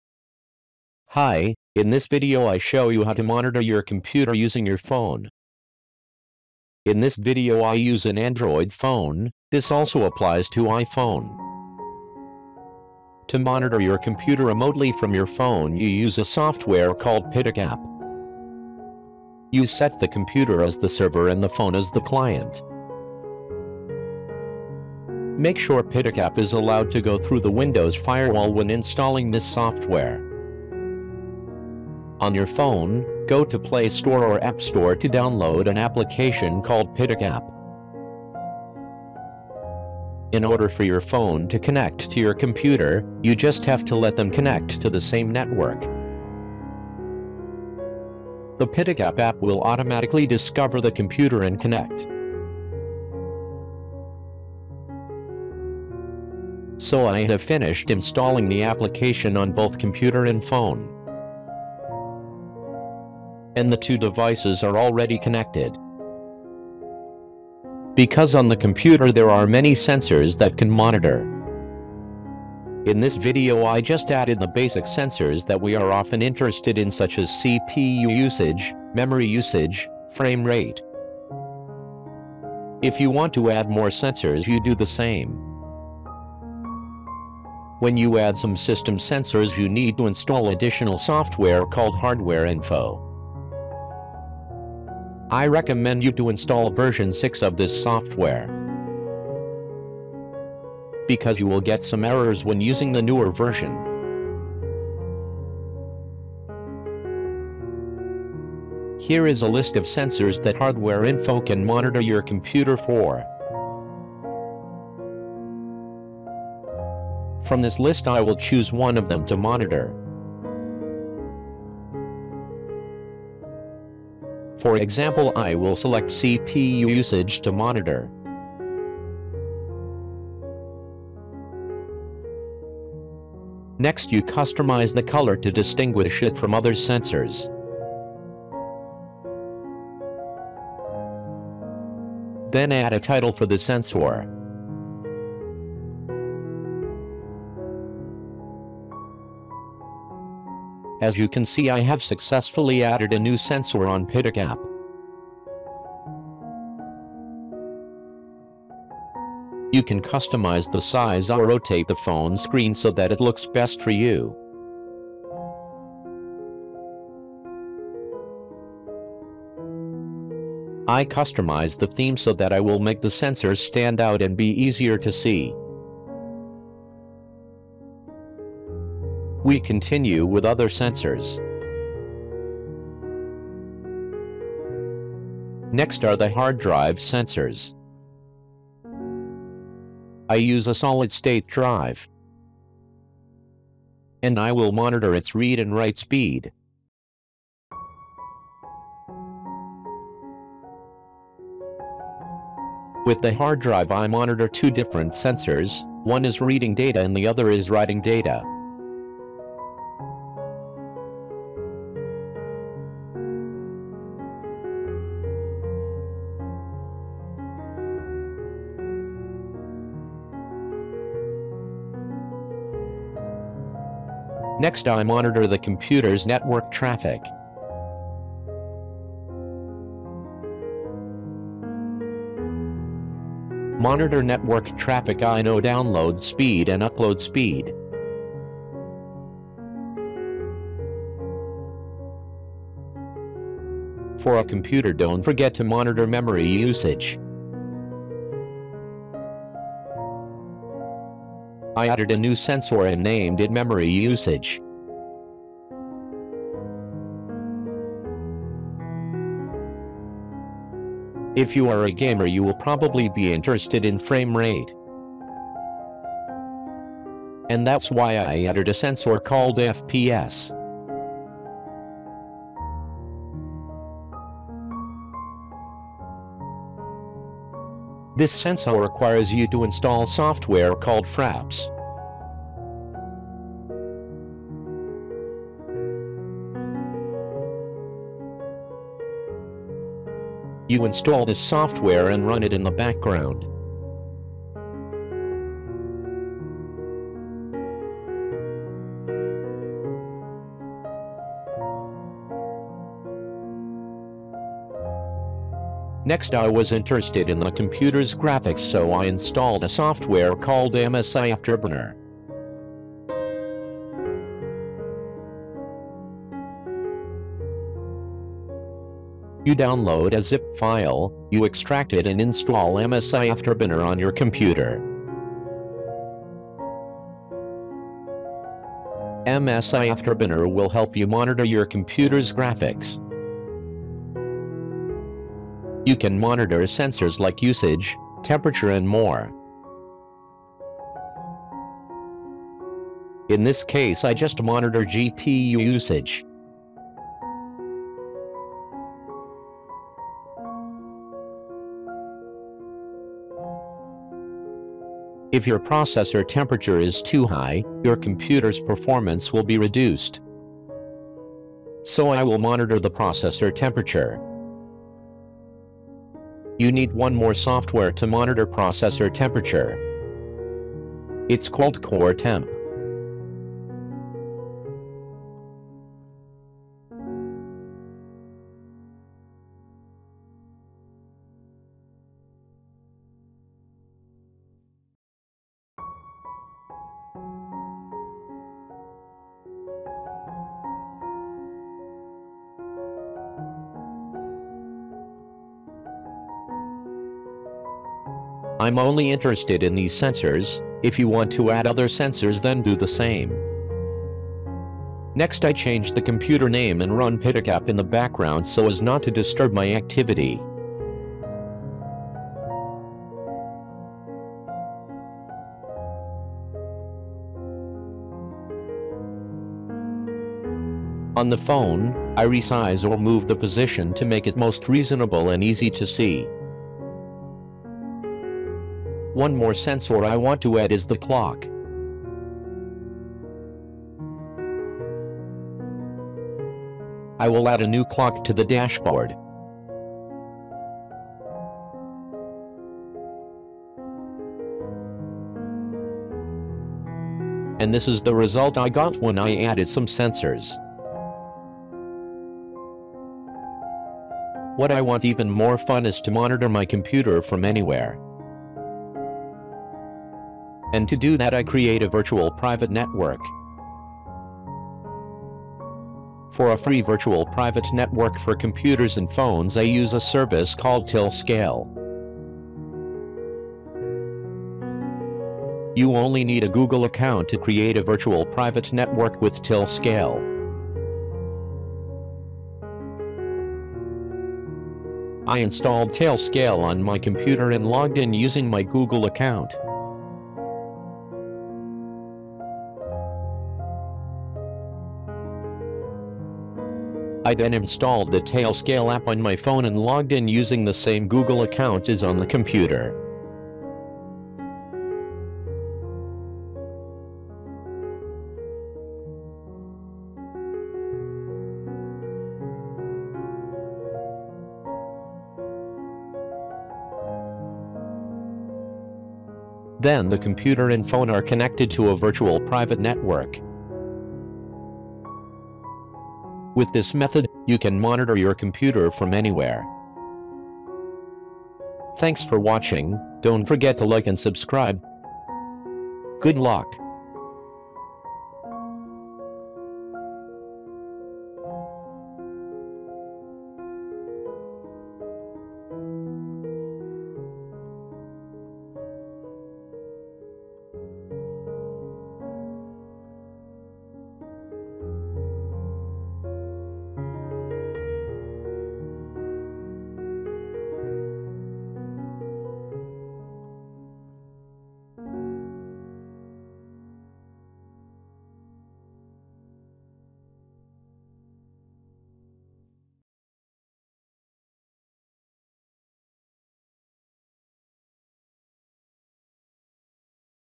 How to monitor your PC from your phone
You can turn your phone into your computer's performance monitor. This video describes step-by-step how to set up your phone as an over-the-air computer performance monitor. You can customize the different sensors you want to monitor. Applies to both android and iOS